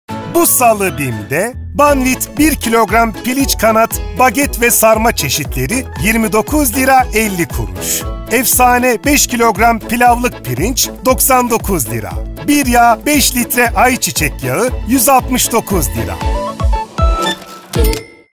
13 Aralık Aktüel- Gıda
Bu Salı BİM'de, Banvit 1 kg Piliç Kanat, Baget ve Sarma çeşitleri 29,50 TL, Efsane 5 kg Pilavlık Pirinç 99,00 TL, Biryağ 5 lt AyçiçekYağı 169,00 TL.